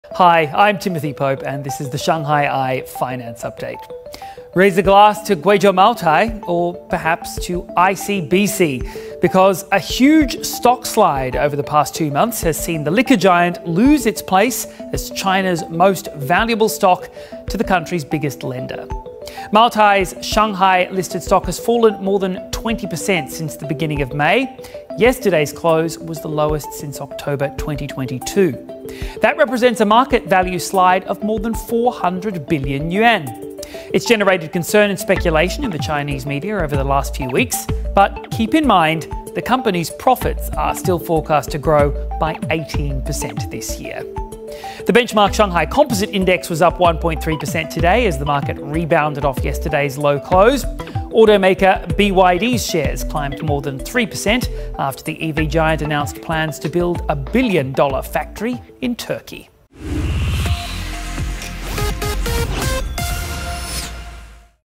Moutai no longer China's most valuable stock
Raise a glass to Kweichow Moutai... or perhaps to ICBC, because a huge stock slide over the past two months has seen the liquor giant lose its place as China's most valuable stock to the country's biggest lender.